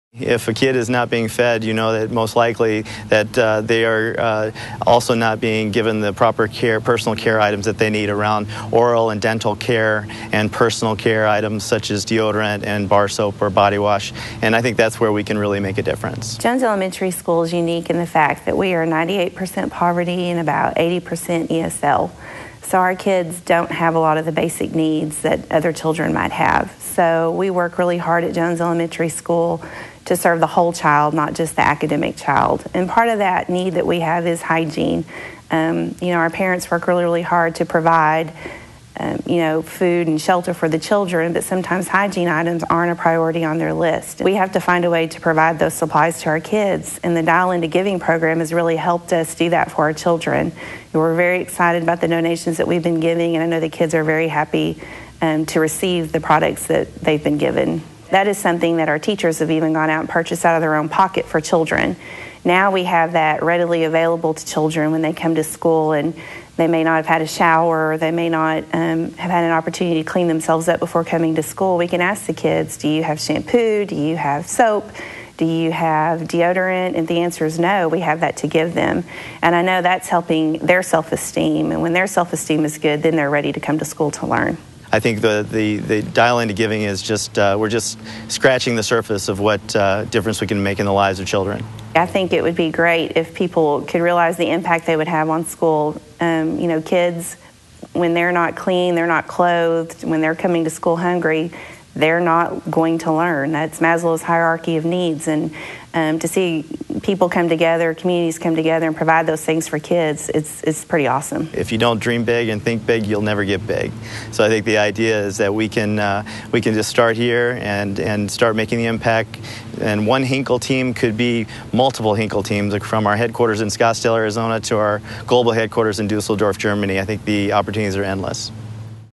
Champions for Kids Simple Service Projects - Henkel, Dial Into Giving
Champions for Kids SIMPLE Service Projects - Henkel, Dial Into Giving. Henkel talks about how their Champions for Kids SIMPLE Service Project, Dial into Giving, will impact kids lives. School teacher, Melissa Fink encouraged by Henkel's support.